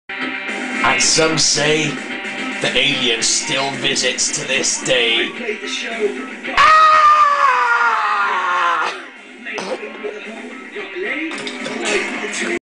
Alien Landing
Alien comes down in the Carsan Twins recording studio!